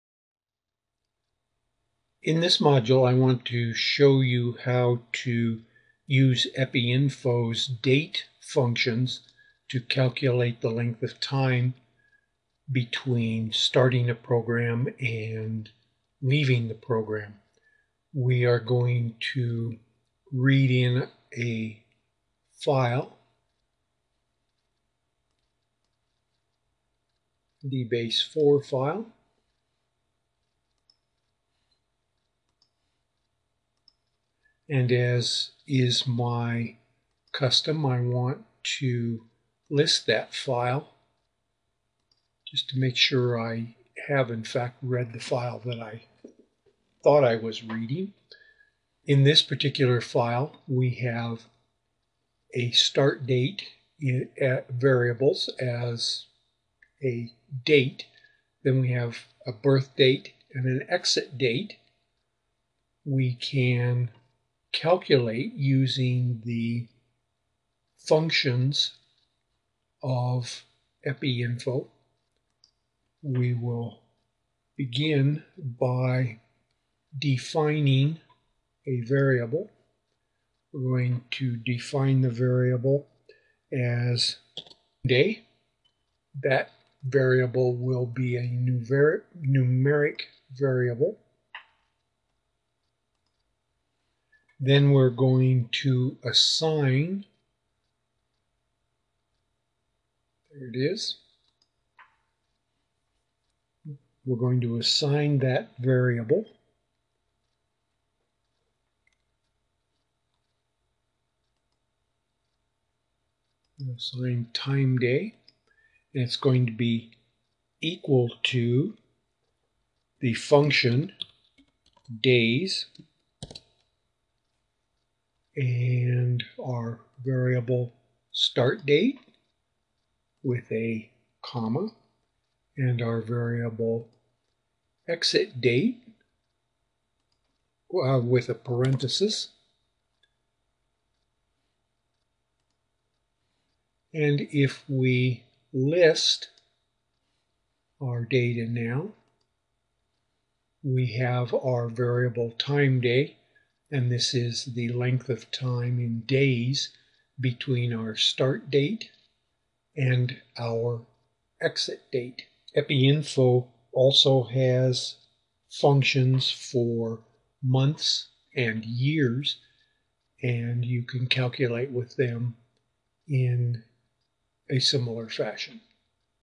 How to use the Epi Info DATES function
This video shows  how  to calculate the number of days between two dates.